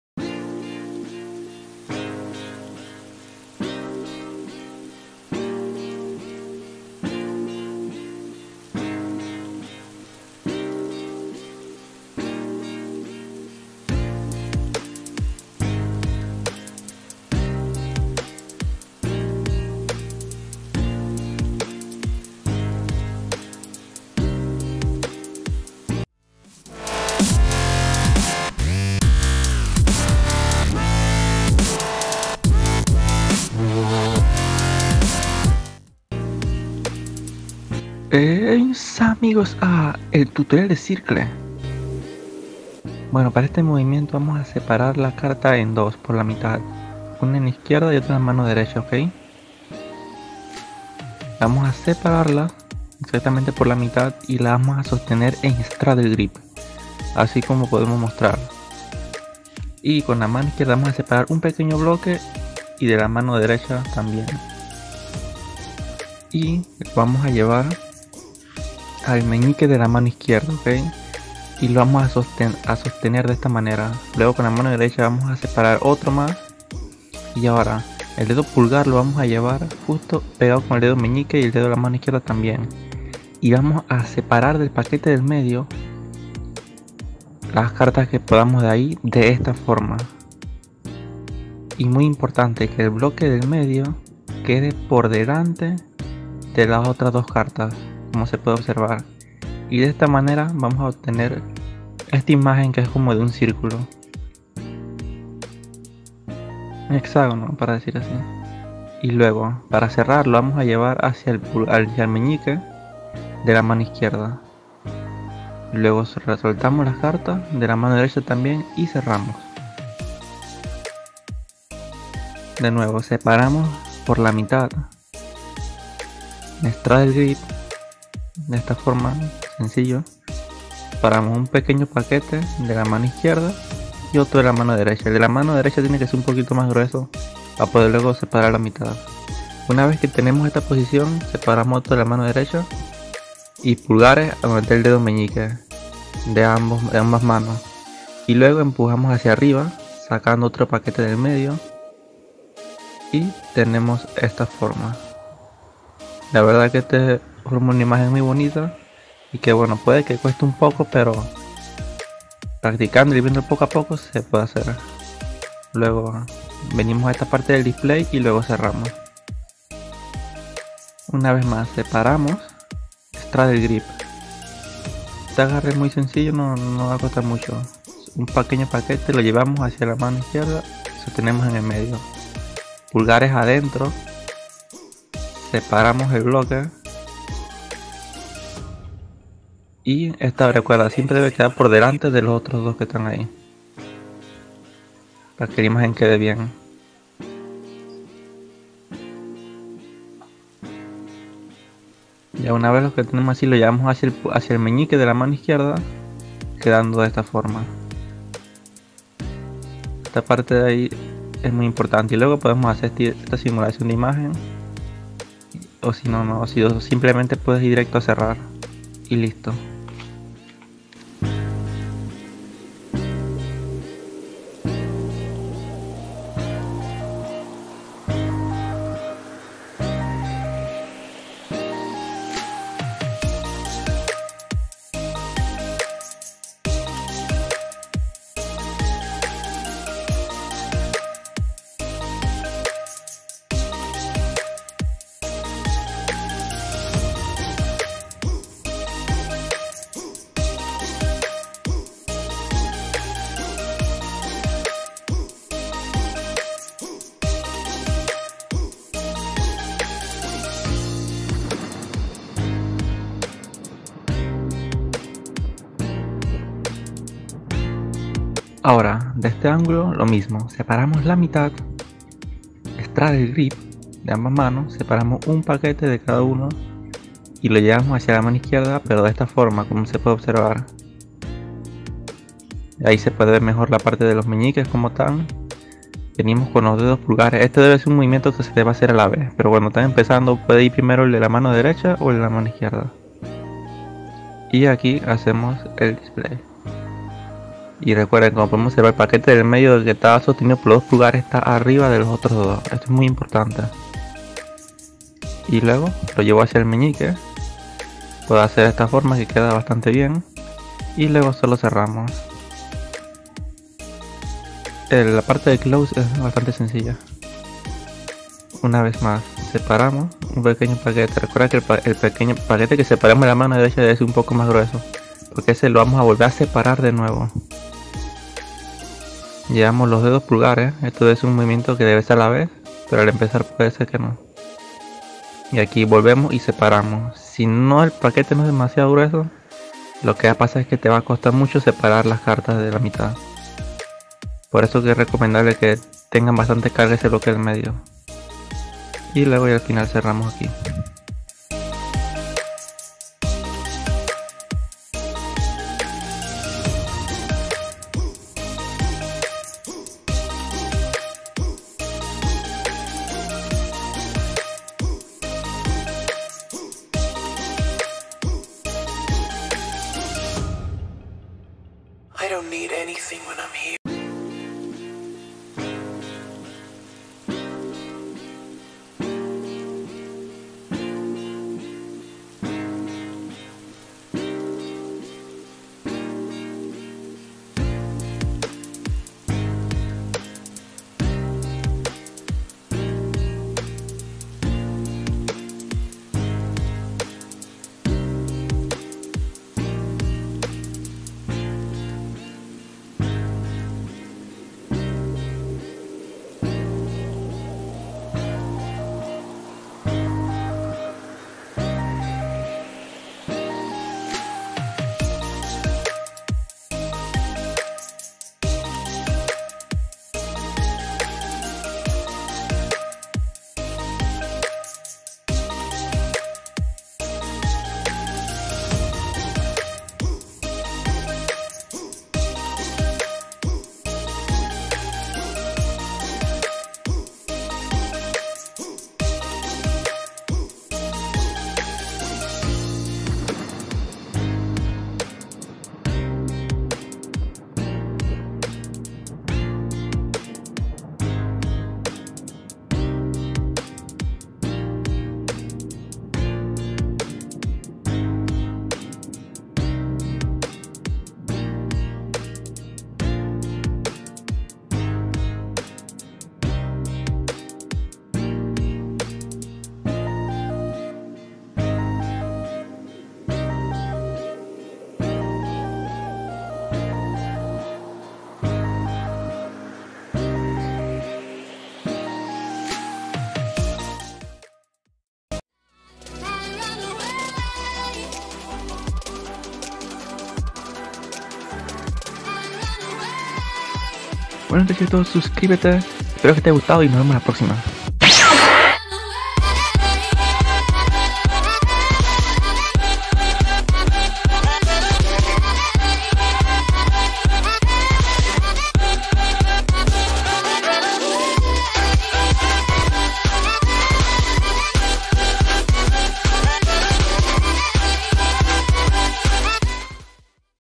CIRCLE / TUTORIAL DE CARDISTRY
Circle es un movimiento que hay que tener sí o sí en el repertorio, es muy llamativo e impresionante